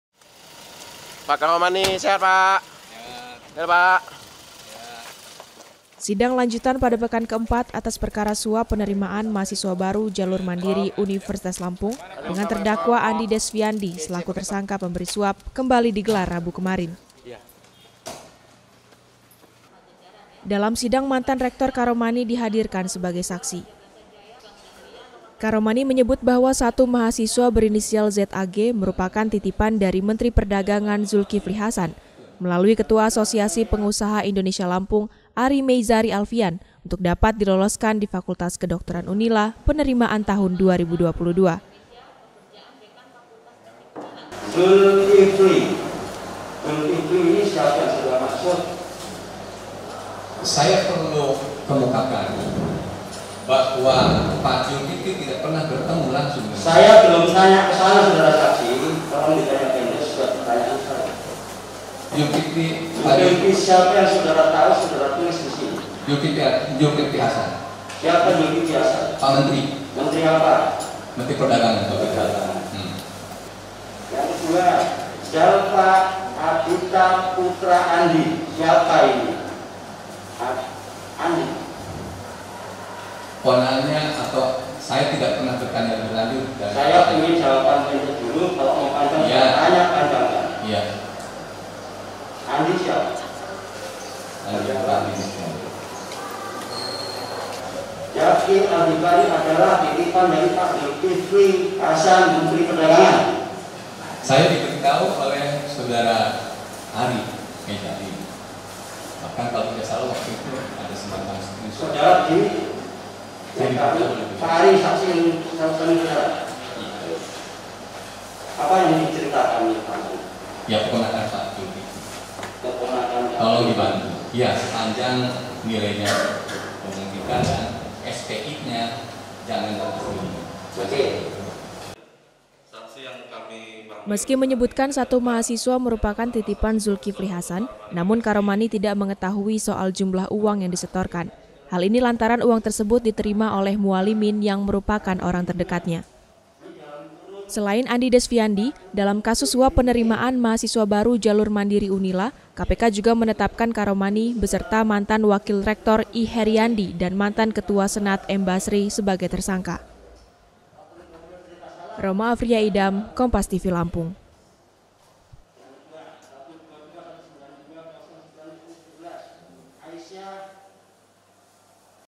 Mantan Rektor Sebut Zulkifli Hasan Titip Keponakan Masuk UNILA
LAMPUNG, KOMPAS.TV - Sidang lanjutan pada pekan ke empat atas perkara suap Penerimaan Mahasiswa Baru Jalur Mandiri Universitas Lampung dengan  terdakwa Andi Desfiandi selaku tersangka pemberi suap kembali digelar rabu kemarin.

Dalam sidang, Mantan Rektor Karomani dihadirkan sebagai saksi.

Karomani menyebut bahwa satu mahasiswa berinisial ZAG merupakan titipan dari Menteri Perdagangan Zulkifli Hasan melalui Ketua Asosiasi Pengusaha Indonesia Lampung, Ary Meizari Alfian untuk dapat diloloskan di Fakultas Kedokteran UNILA penerimaan tahun 2022.

"Ada Zulkifli Hasan dan Andi Desfiandi," ujar Karomani sebagai saksi dan tersangka penerima suap.

Meski menyebutkan satu mahasiswa merupakan titipan Zulkifli Hasan, namun Karomani tidak mengetahui  soal jumlah uang yang disetorkan, hal ini lantaran uang tersebut diterima oleh Mualimin yang merupakan orang terdekatnya.

Selain Andi Desfiandi, dalam kasus suap Penerimaan Mahasiswa Baru Jalur Mandiri UNILA, KPK juga menetapkan Karomani beserta Mantan Wakil Rektor I Heriyandi dan Mantan Ketua Senat M Basri sebagai tersangka.